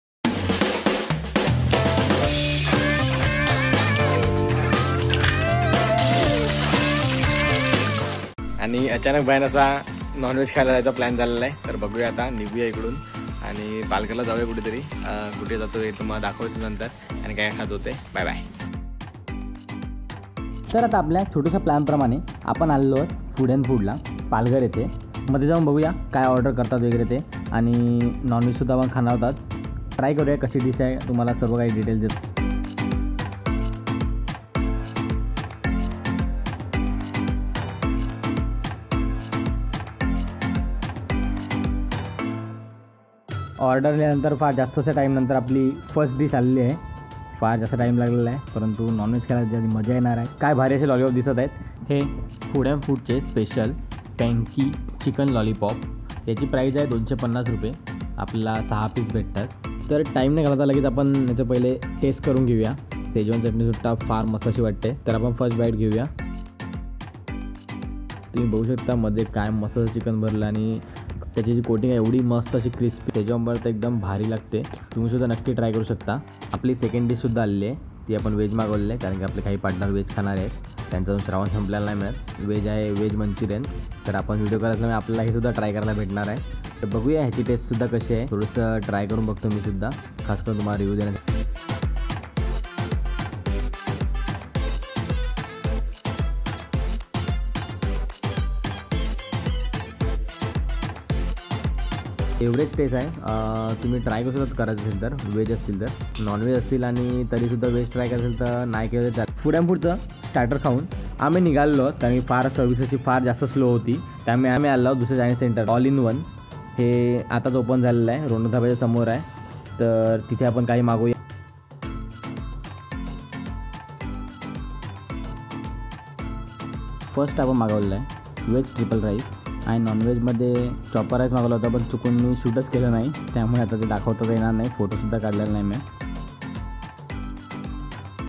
Cousin सोबत केली पालघर मध्ये पार्टी | Food & Food | All In One | Jugadu Chef
Panky Chicken Lolipop | Veg Manchurian | Food & Food | All In One | Jugadu Chef